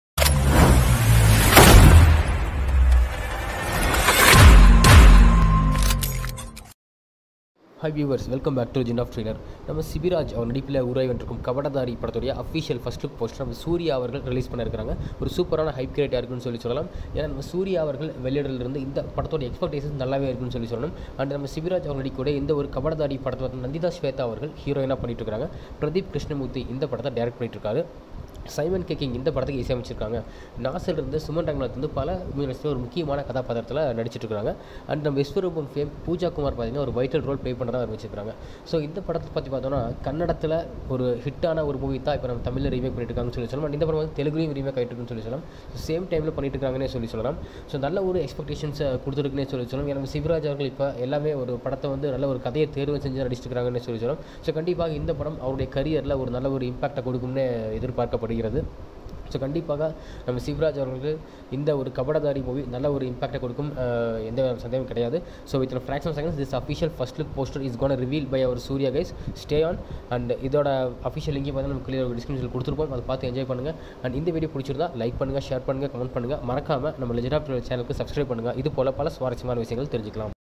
Kabadadaari First Look | Sibiraj | Nandita Swetha | Tamil Movie 2020 | Update
Exclusive update about Tamil Movie Kabadadaari First Look Release